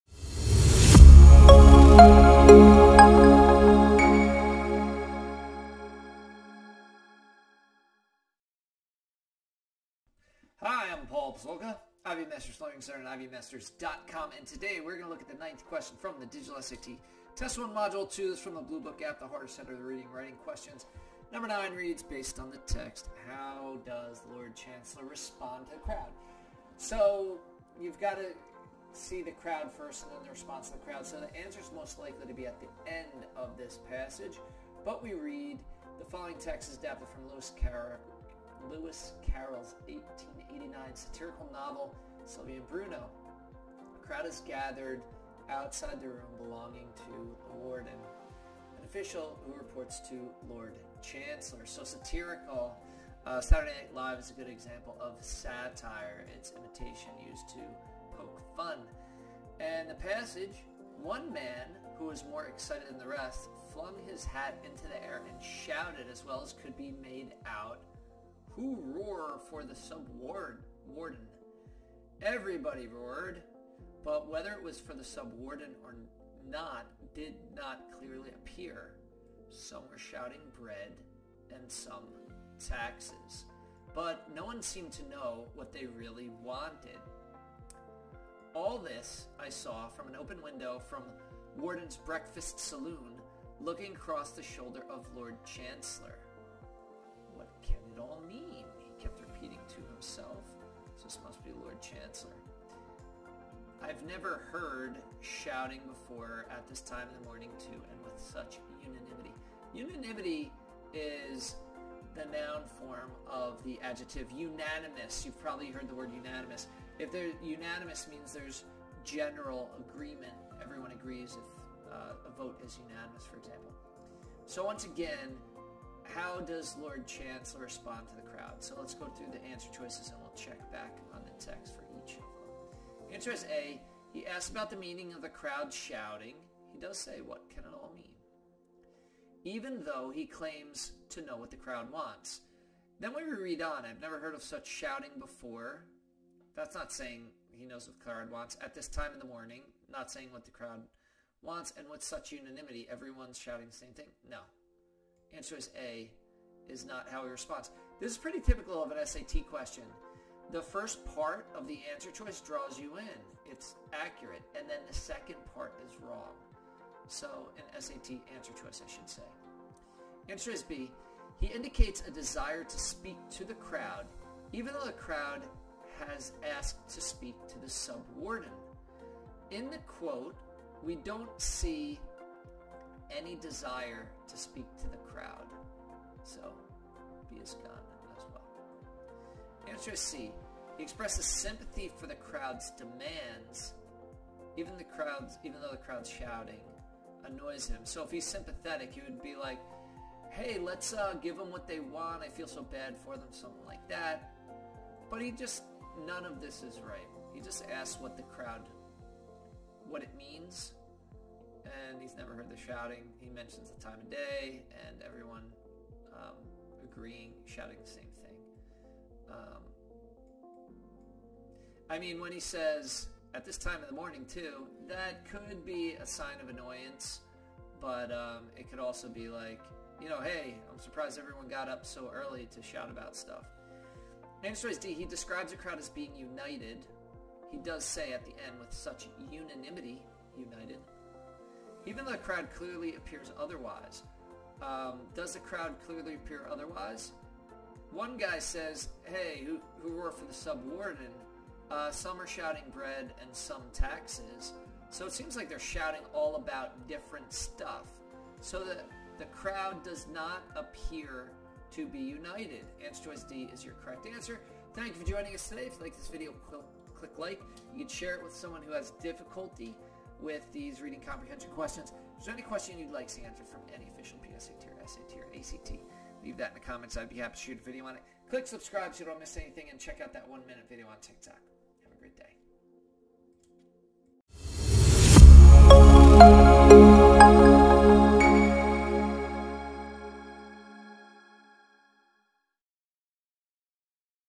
Digital SAT practice test 1 module 2 number 9 harder set reading and writing || Ivy Masters
A typical type of tricky wrong answer choice

Reading comprehension, Blue book app, digital SAT, Bluebook app, New SAT

The following text is adapted from Lewis Carroll's 1889 satirical novel Sylvie and Bruno.  A crowd has gathered outside a room belonging to the Warden, an official who reports to the Lord Chancellor.

One Man, who was more excited than the rest, flung his hat high into the air, and shouted (as well as I could make out) "Who roar for the Sub-Warden?" Everybody roared, but whether it was for the Sub-Warden, or not, did not clearly appear: some were shouting "Bread!" and some "Taxes!", but no one seemed to know what it was they really wanted.
All this I saw from the open window of the Warden's breakfast-saloon, looking across the shoulder of the Lord Chancellor.
"What can it all mean?" he kept repeating to himself./  "I never heard such shouting before-- and at this time of the morning, too! And with such unanimity!"

Based on the text, how does the Lord Chancellor respond to the crowd?

(A) He asks about the meaning of the crowd's shouting, even though he claims to know what
      the crowd wants.
(B) He indicates a desire to speak to the crowd, even though the crowd has asked to speak to
      the Sub-Warden.
(C) He expresses sympathy for the crowd's demands, even though the crowd's shouting
      annoys him.
(D) He describes the crowd as being united, even though the crowd clearly appears otherwise.